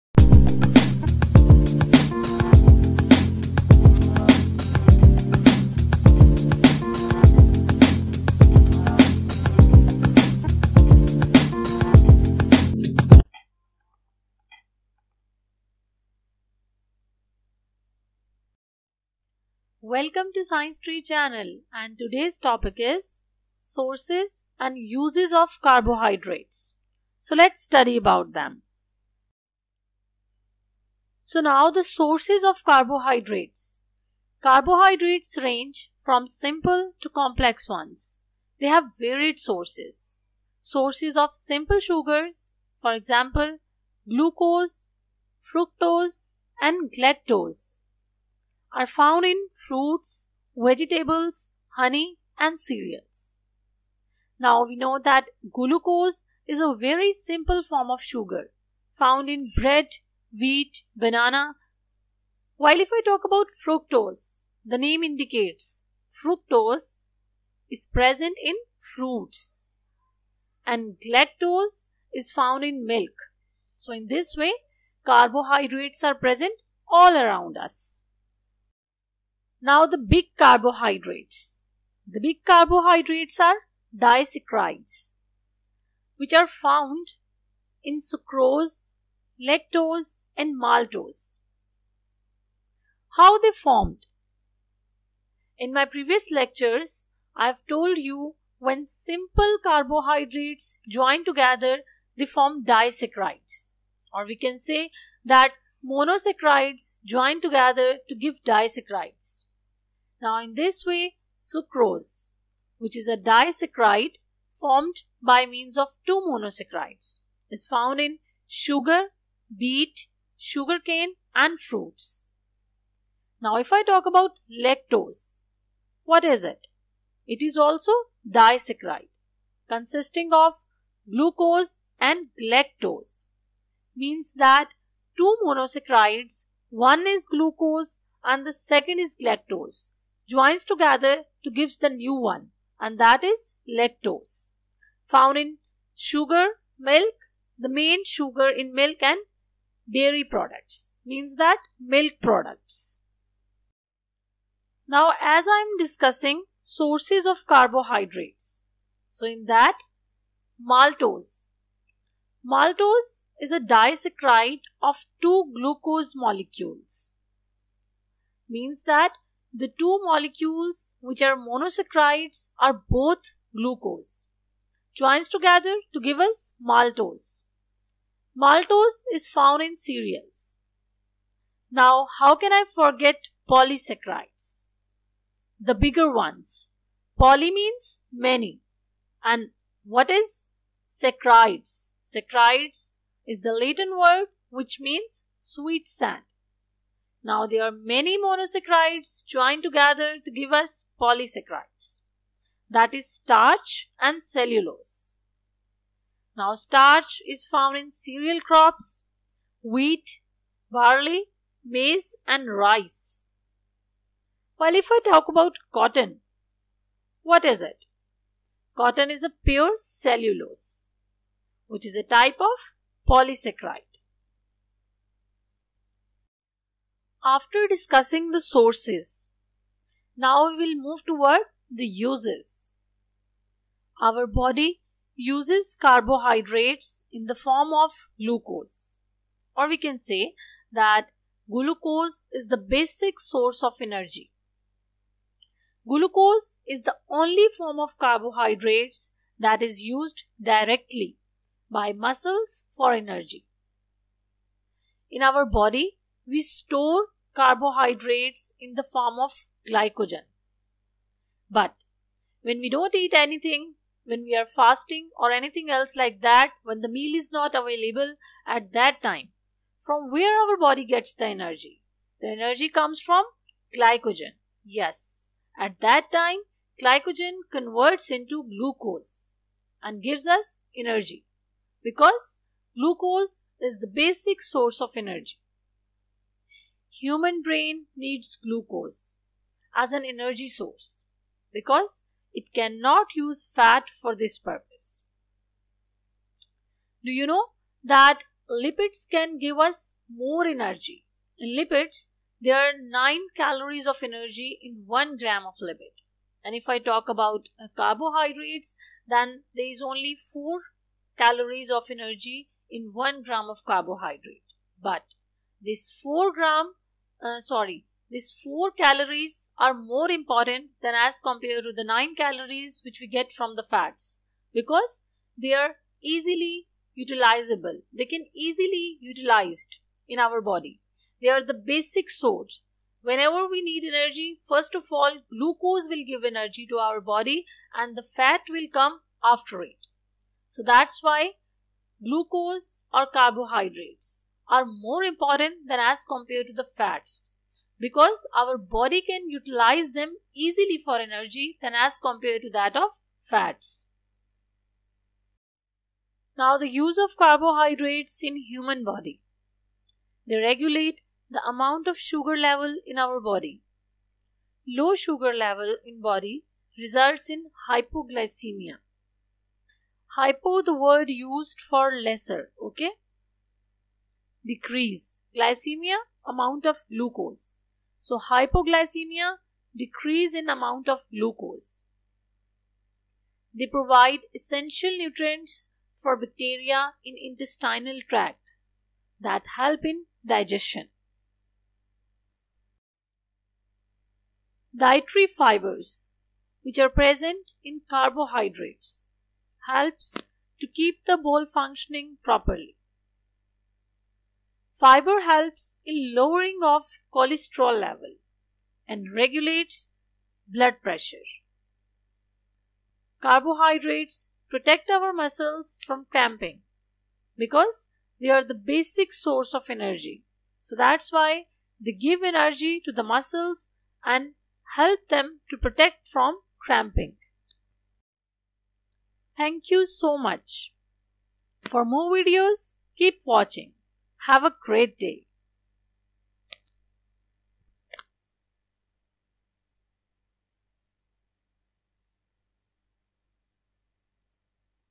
Sources & Uses of Carbohydrates | Biochemistry | Carbohydrates Uses in Human Body || Science Tree
This video covers Sources & Uses of Carbohydrates 10th Class Chapter 13 (Biochemistry). The topic is divided into two parts. In first part we will learn about Sources of Carbohydrates (Simple to Complex ones) and in second part we will learn about uses of Carbohydrates, their uses in Human body.

1. Sources & Uses of Carbohydrates
2. Sources of simple sugar
3. Sources of complex carbohydrates
4. Uses of Carbohydrates
5. Uses of Carbohydrates in Human body
6. Carbohydrates are essential nutrients for bacteria in intestinal tract that help in digestion.
7. Regulate the amount of sugar level
8. Dietary fibre helps bowel functioning properly
9. Protect our muscles from cramping